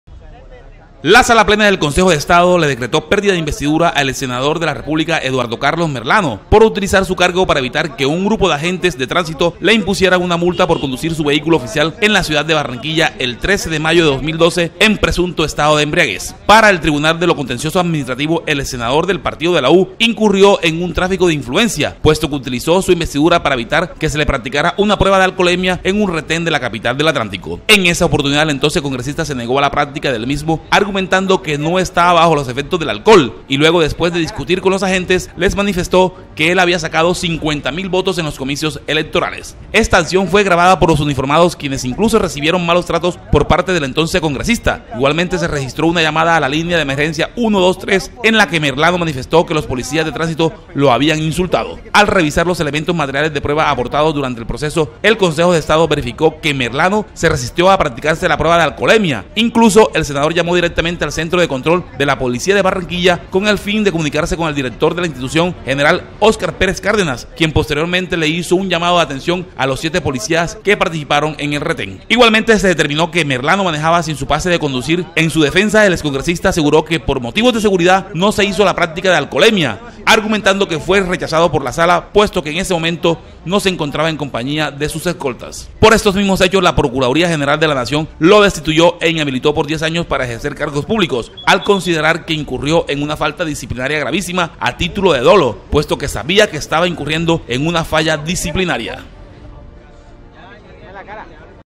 EX SENADOR MERLANO CON MUERTE POLÍTICA
Decretan muerte política a Merlano. La Sala Plena del Consejo de Estado decretó la pérdida de investidura por tráfico de influencias al ex senador Eduardo Carlos Merlano. Para los consejeros, Merlano antepuso su condición de senador sobre un control de la Policía al negarse en mayo del 2012, a la práctica de una prueba de alcoholemia que le reclamaron patrulleros de la Policía en Barranquilla.